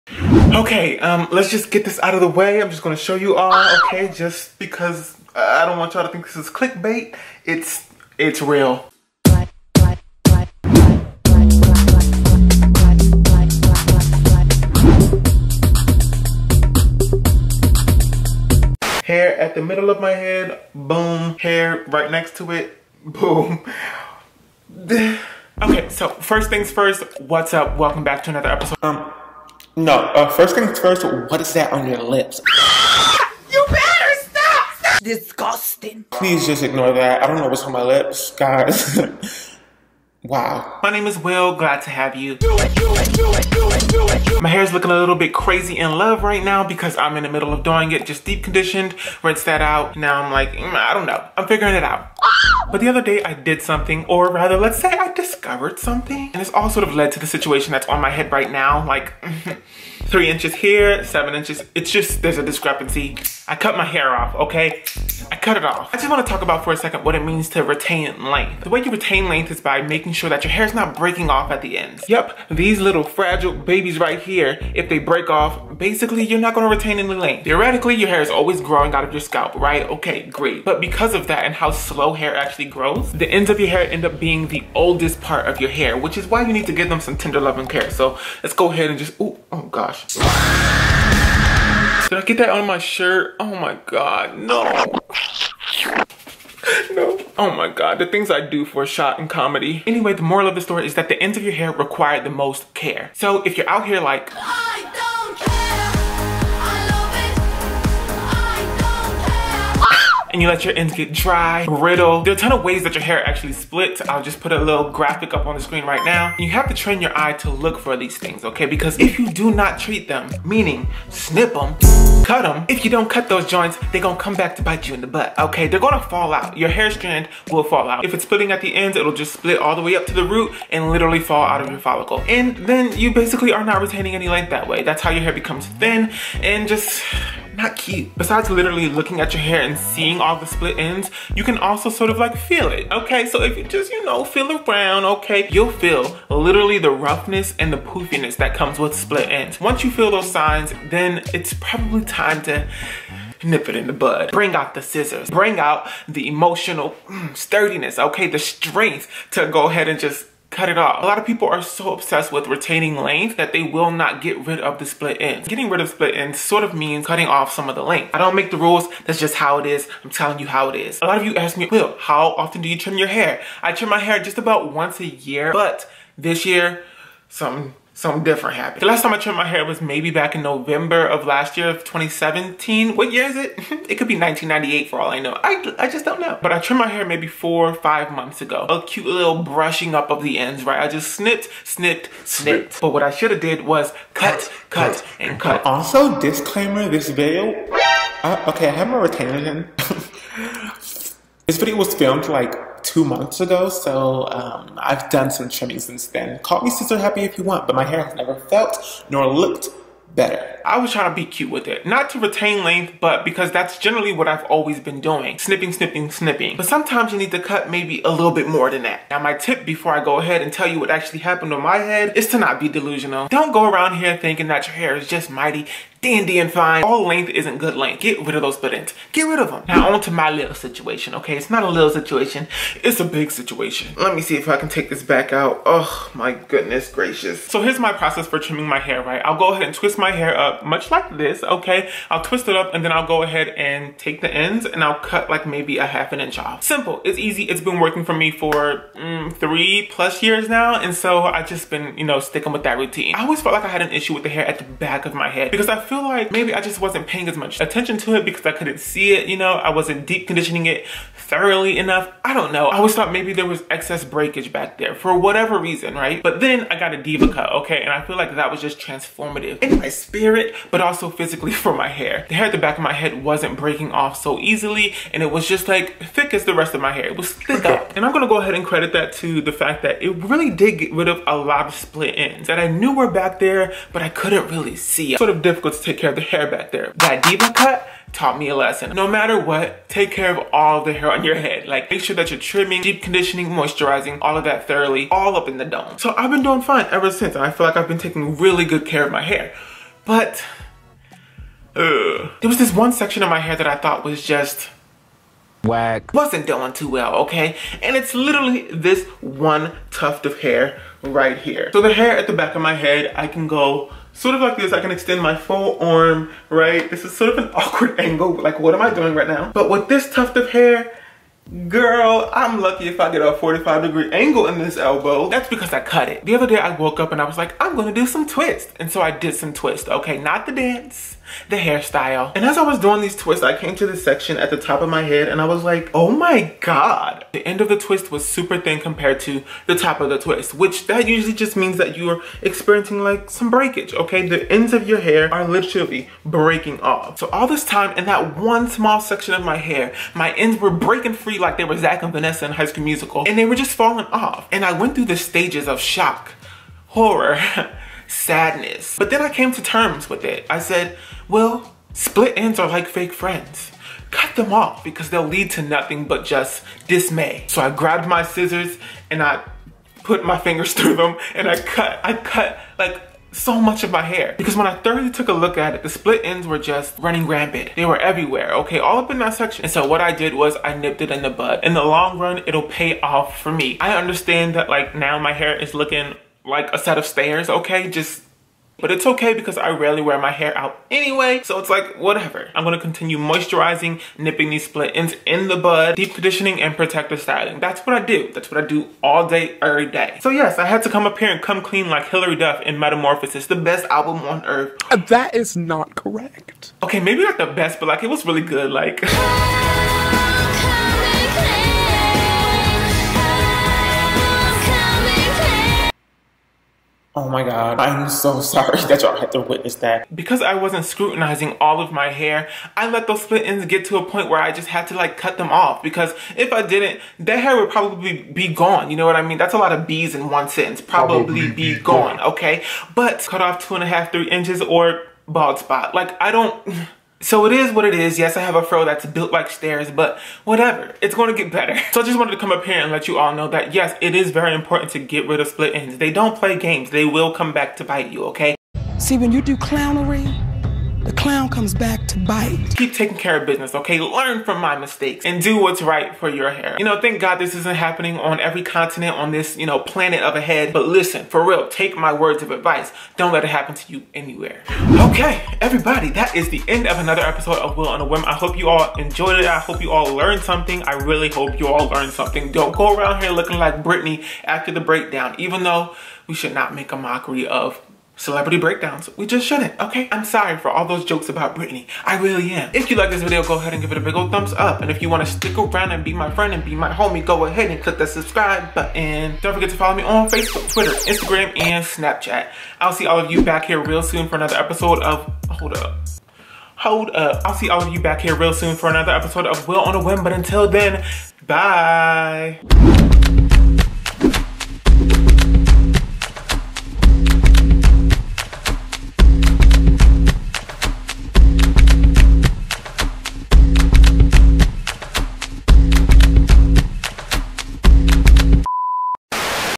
MY SPLIT ENDS DAMAGE & how to avoid it.
Learn from my mistakes y'all.

Split ends are not a myth. They are real and will snatch you up when you least expect it. Especially if they reside in a part of your head that you can barely see. 😩

Split ends happen when the protein in your hair strands experiences damage. Manipulating dry hair, not wearing your silk/satin scarf to bed, lack of protective styles, flat ironing your hair too often, and chemical processing can weaken your hair and cause it to split.

Take care of your hair.  Routinely moisturize, deep condition, and protective style to strengthen it from the inside out and you should be fine.

Split ends are like fake friends...cut them off.

As always!
Much Love ❤️
Will